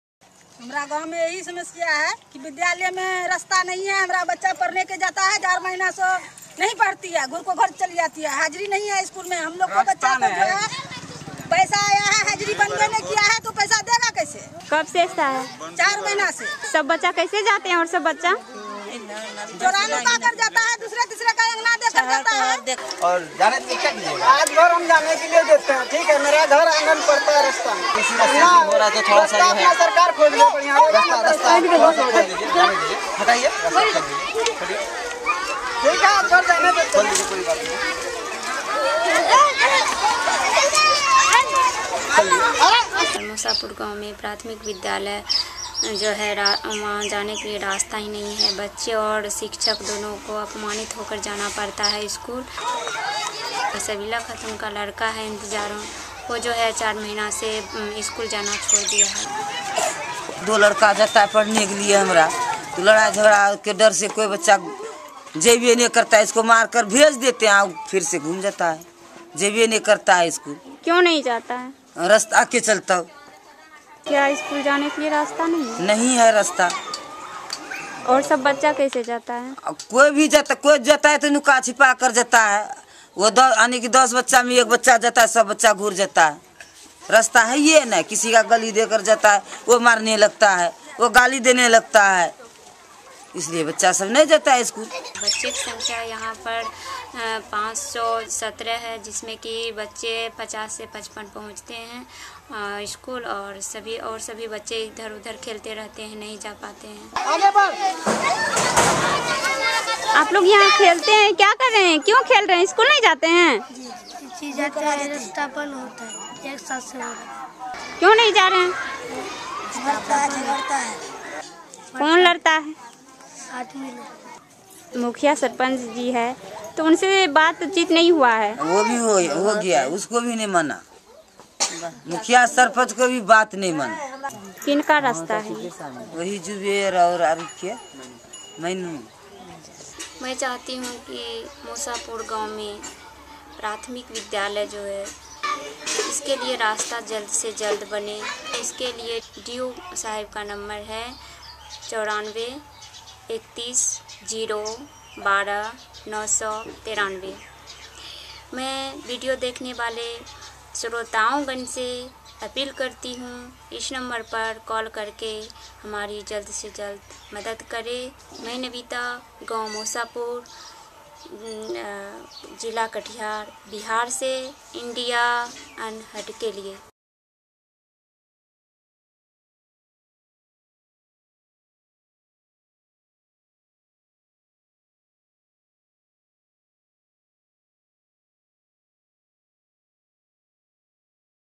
Better Roads To Increase School Enrollment
VV-PACS Community Correspondent Navita Devi from Katihar district, Bihar reports on a tricky situation. The village has a Primary school but there is no road leading to it. The children go to school, passing by compound of the village residents, often getting insulted by the house owners.

Call to Action: You can help by calling the District Education Officer, Katihar  at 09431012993 and pressuring him to build a road leading to the Musapur Primary school.